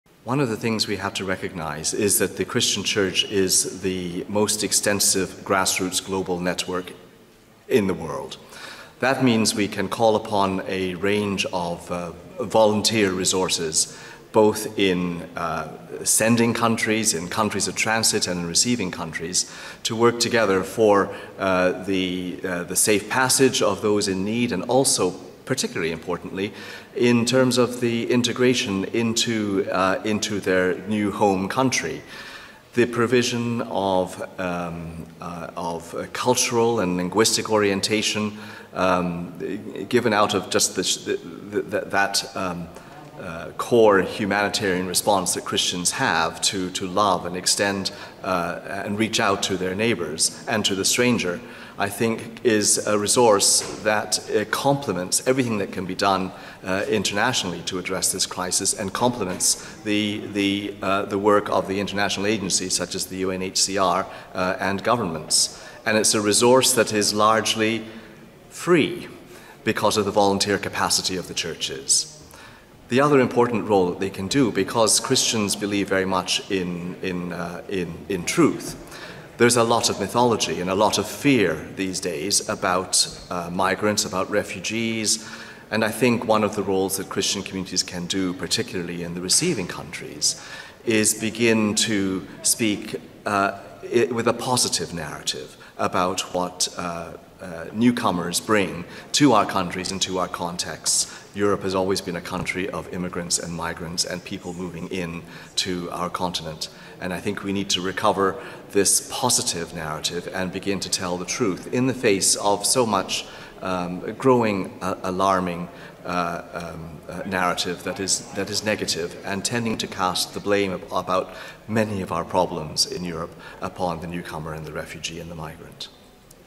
The Christian Church can help promote a more positive narrative about migration
Bishop David HAMID, Church of England’s Diocese in Europe – UK